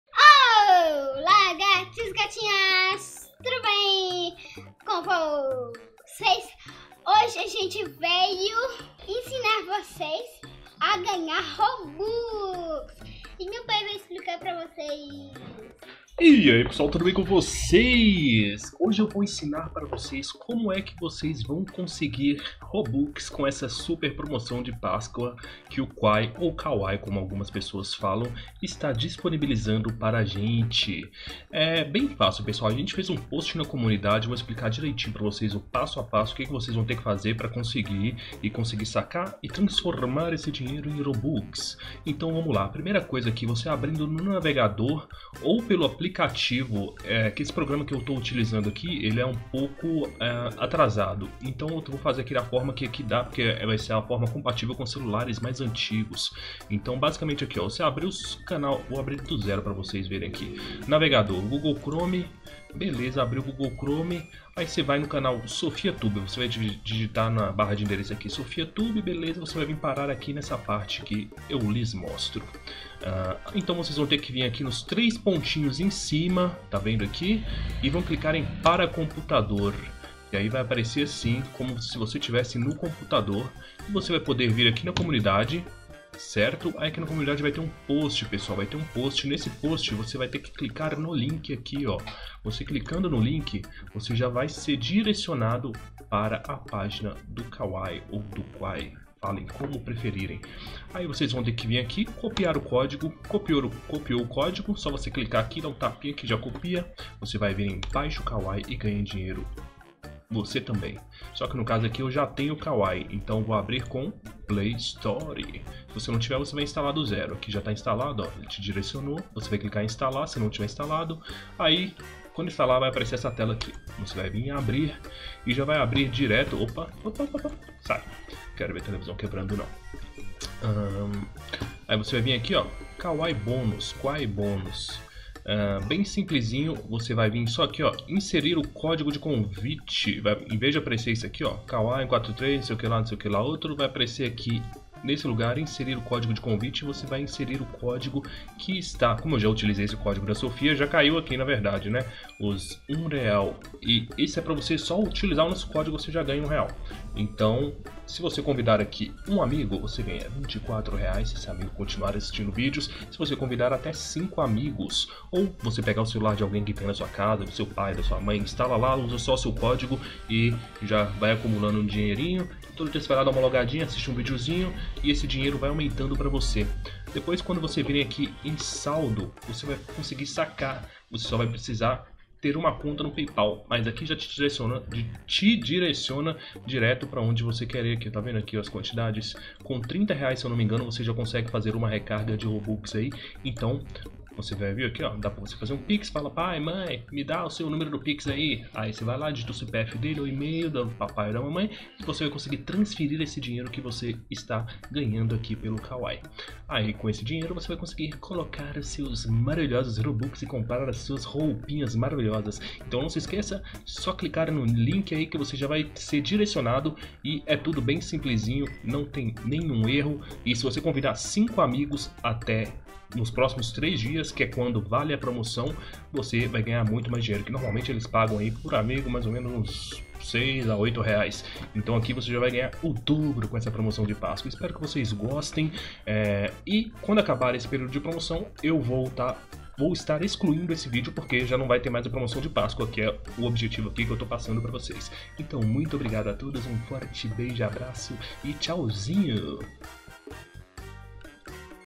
COMO GANHAR ROBUX🤑 PASSO A PASSO, SUPER PROMOÇÃO DE PASCOA!!
💜Olá Gatinhos e Gatinhas, No video de hoje Vamos ensinar para vocês como conseguir robux, sem sair de casa, basta seguir o passo a passo.

1. Clique no link, copie o meu código de convite, baixe e instale o Kwai.
2. Vincule meu código de convite para receber dinheiro imediatamente e continue ganhando dinheiro usando-o todos os dias!